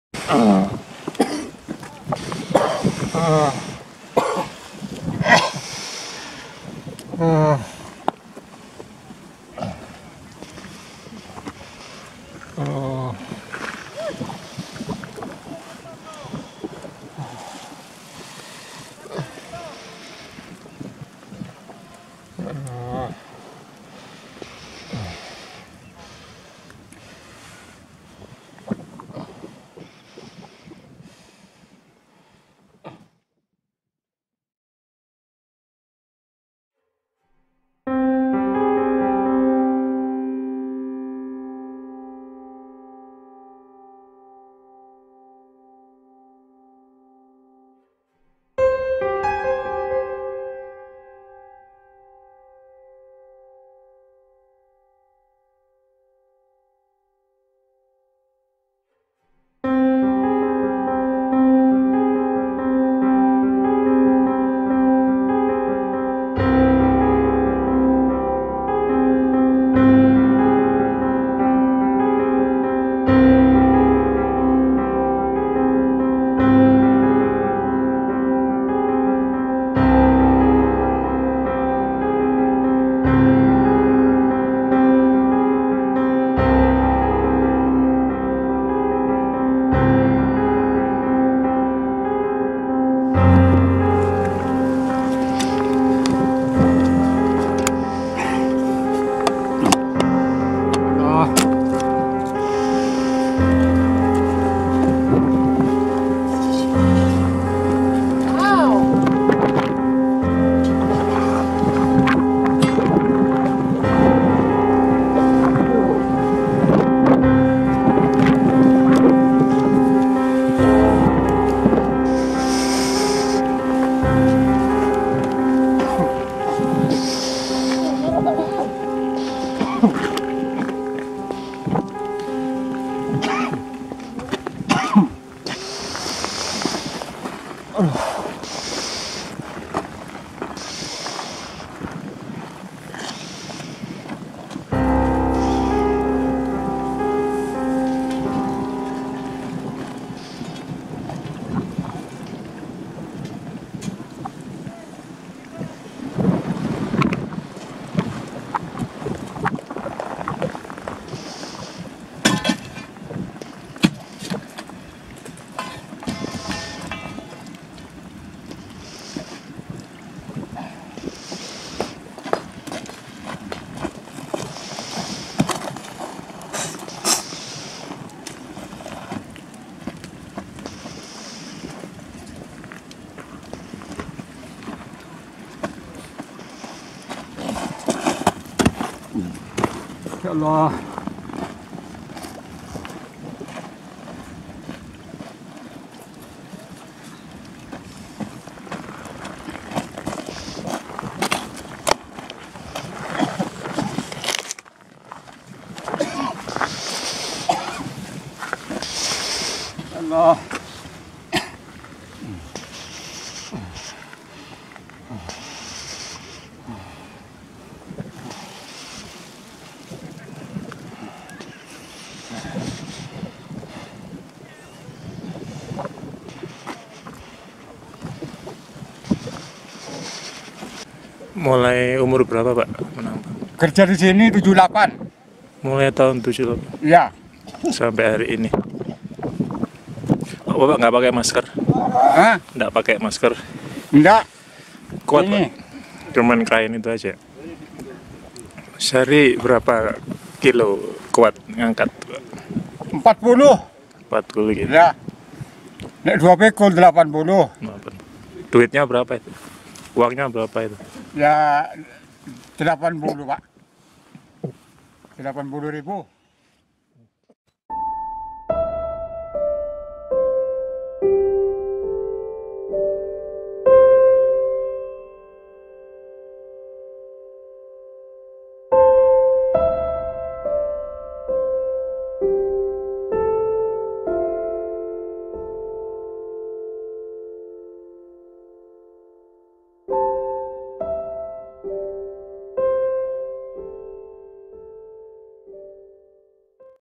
FILM PENDEK I PENAMBANG PEMBERANI I INDONESIA
Video dokumenter pendek :

Mosana, 70, salah satu penambang belerang di kawah Ijen, Banyuwangi, Jawa Timur. Bekerja sejak tahun 1978 hingga video ini dibuat (2008). Mosana bekerja tanpa masker dan hanya menggunakan kain yang sesekali dibasahi air, meski anjuran menggunakan masker bagi penambang tradisional sudah dikampayekan .

Salam hangat :)